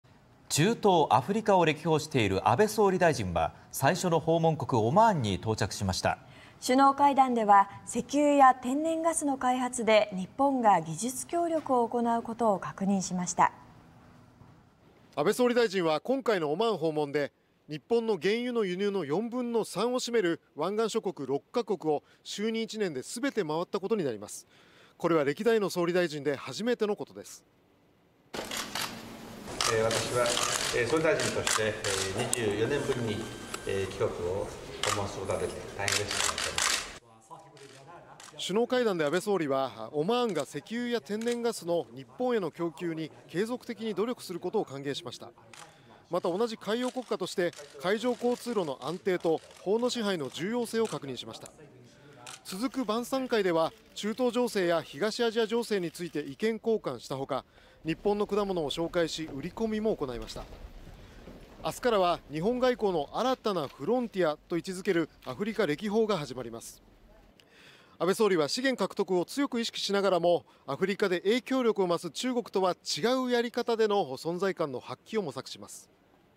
石油、天然ガス開発で技術協力 日・オマーン首脳(14/01/10)
中東・アフリカを歴訪している安倍総理大臣は、最初の訪問国のオマーンに到着しました。首脳会談では、石油や天然ガスの開発で日本が技術協力を行うことを確認しました。